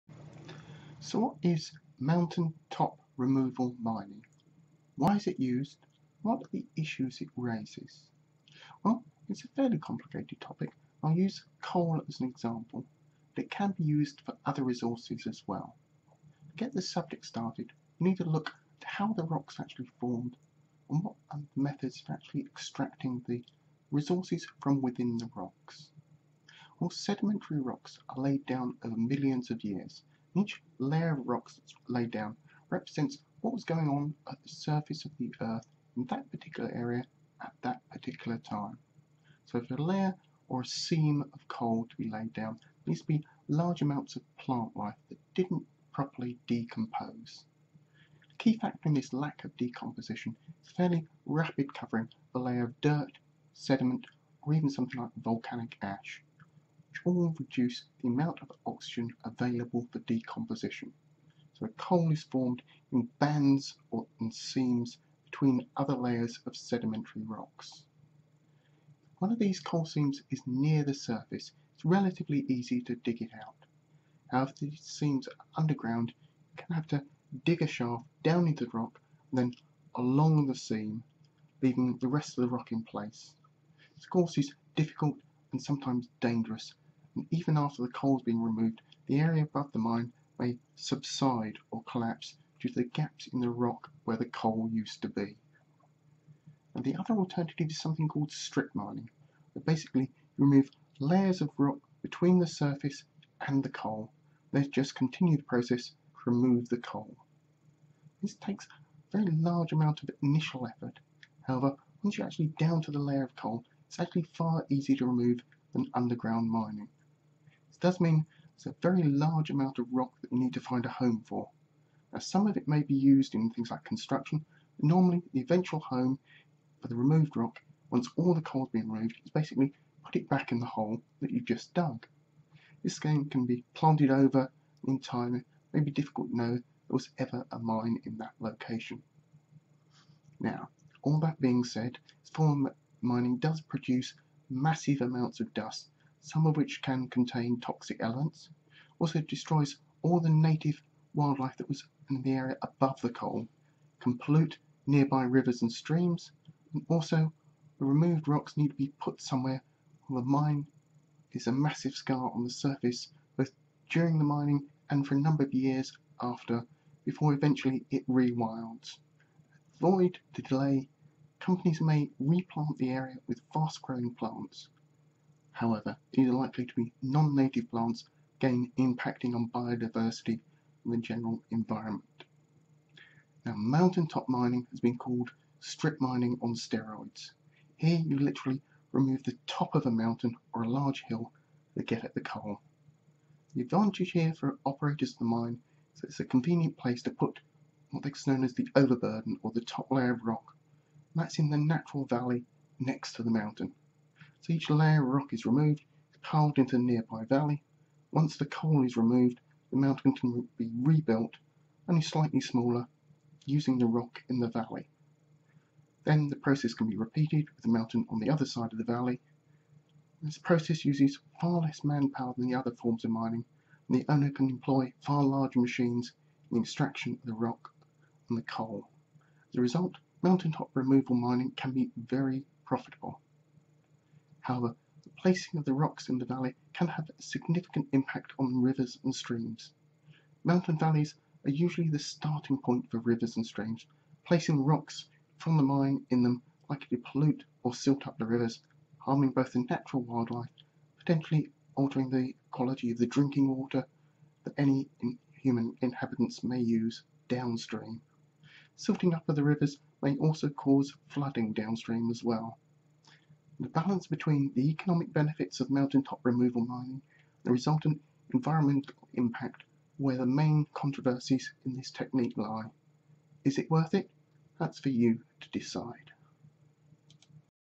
What is mountaintop removal mining?
Mountaintop removal mining is a version of strip mining where the whole mountain is removed to get at the layers or seams of coal or other resources underneath. However this method of mining is controversial due to the environmental damage and hazards that result.